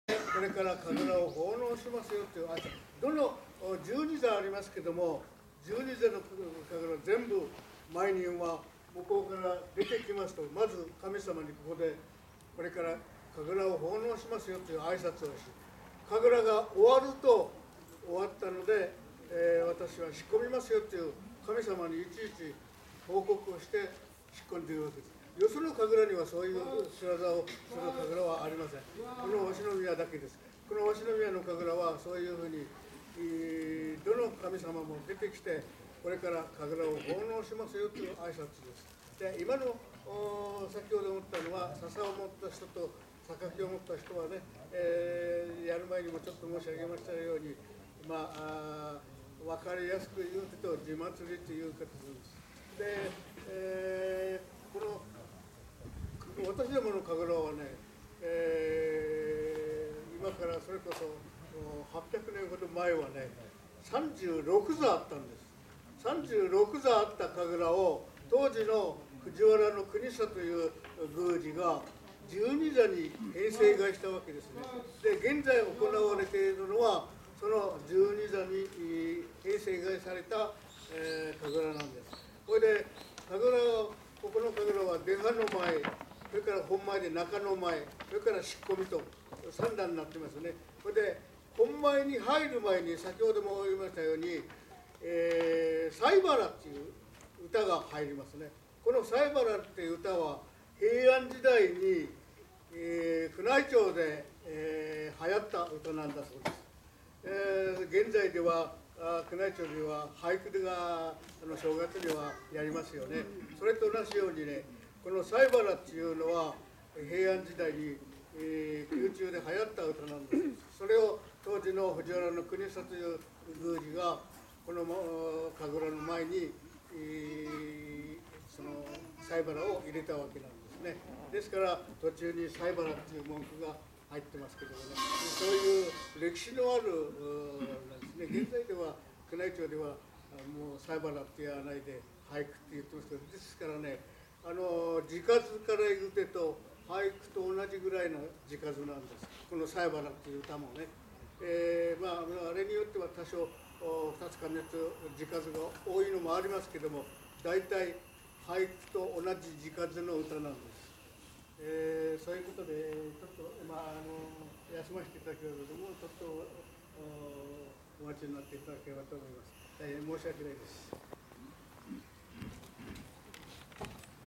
鷲宮催馬楽神楽 解説 Washinomiya saibara kagura explanation(Japanese)
Washinomiya Saibara Kagura　⇒

Washinomiya Shrine　⇒

鷲宮催馬楽神楽（わしのみやさいばらかぐら）

催馬楽神楽の演目は、基本となる12座があり、その他に3座があります。

催馬楽神楽の演目　より転載　⇒

2020年2月14日金曜日
埼玉県久喜市
鷲宮神社　年越祭　にて撮影

鷲宮神社　⇒